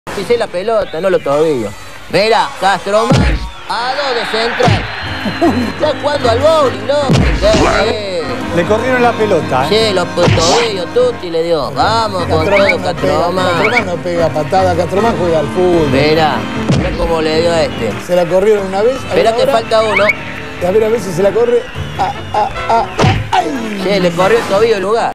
CASTROMAN RASPA | Rosario Central 1 Vs Velez 1 | Clausura 2005
Fecha: 11ª
30 de abril de 2005
Estadio: Rosario Central
Árbitro: Ángel Sánchez

ROSARIO CENTRAL   1

Juan Ojeda; Paulo Ferrari, Ronald Raldes, Ramiro Fassi, Germán Rivarola; Hernán Encina, Leonardo Borzani, Mauro Monges (66´Damián Ledesma), Emiliano Papa; Pablo Vitti (61´Marco Ruben), Emanuel Villa (81´Germán Alemanno).
DT: Ángel Tulio Zof.

VÉLEZ SARSFIELD   1

Gastón Sessa; Santiago Ladino, Fabricio Fuentes, Maximiliano Pellegrino, Ariel Broggi; Jonás Gutiérrez, Leandro Somoza, Marcelo Bravo (89´Maximiliano Bustos), Leandro Gracián (80´Emanuel Centurión); Lucas Castromán, Juan Manuel Martínez (67´Federico Arias).
DT: Miguel Ángel Russo.

Goles: ST: 41´Germán Alemanno, 44´Lucas Castromán.
Amonestados: PT: 43´Emiliano Papa; ST: 27´Santiago Ladino
Expulsados: ST: 45´Lucas Castromán.